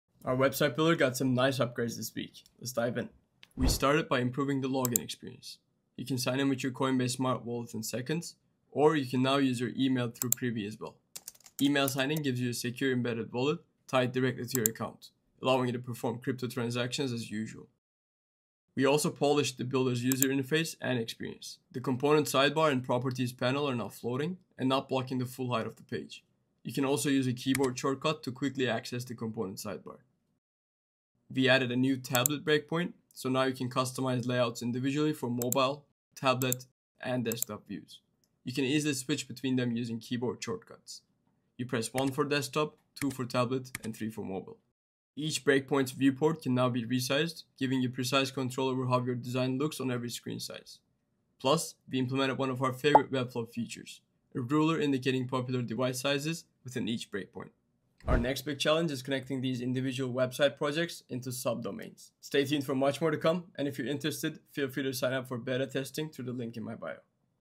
Some big upgrades to our website builder platform!
Big upgrades this week for our blockchain-integrated website builder:

🔹 Instantly sign in with Coinbase Smart Wallet or via email (powered by Privy).
🔹 Floating panels, keyboard shortcuts, and tablet breakpoint added for better responsive designs.
🔹 Resize your viewport and precisely control how your site looks on every device.